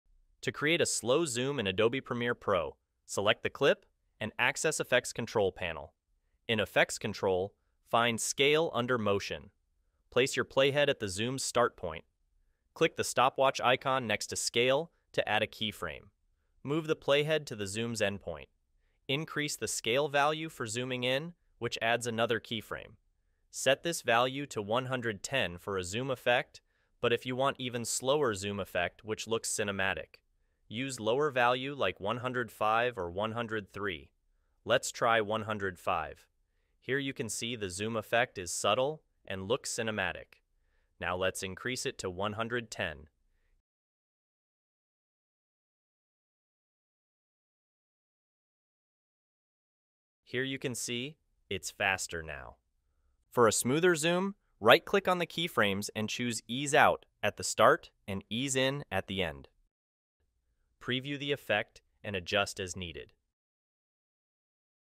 How to SLOWLY ZOOM IN on premiere pro (Easy Way)
How to SLOWLY ZOOM IN on premiere pro.
Creating a slow zoom effect in Adobe Premiere Pro is an essential skill for video editors, allowing you to draw attention to specific parts of your footage, add dramatic emphasis, or simply enhance the visual storytelling of your project. Adobe Premiere Pro, a leading software in the field of video editing, offers a range of tools and features that make achieving this effect both straightforward and customizable.

The slow zoom effect is particularly effective in a variety of video projects. Whether you're working on a documentary, a feature film, a music video, or a personal project, the ability to zoom in slowly on a subject can significantly impact your audience's viewing experience. This technique can be used to highlight important details, create a sense of intimacy, or build suspense.

Adobe Premiere Pro's keyframing capabilities are central to creating this effect. Keyframes allow you to smoothly transition between different zoom levels, giving you control over the timing and the degree of the zoom. This flexibility is part of what makes Premiere Pro a preferred choice for video editors.

Moreover, the slow zoom effect is not just about the visual impact; it also contributes to the storytelling aspect of your video. By focusing closely on a subject or a particular part of the scene, you can direct the viewer's attention and convey a specific mood or message. This can be particularly powerful in combination with other editing techniques, such as music, lighting, and color grading, to create a cohesive and compelling narrative.